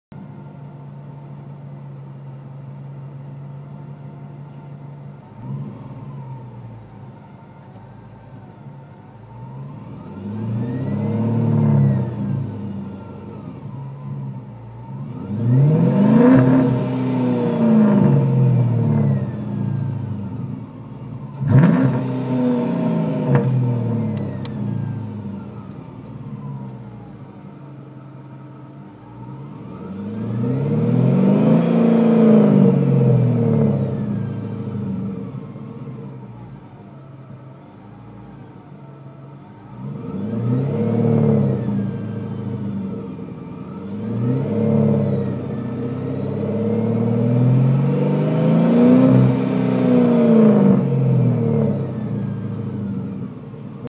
Charger S/C SLP Stage 2 Catless Mids
Just Revving the engine, listen to that super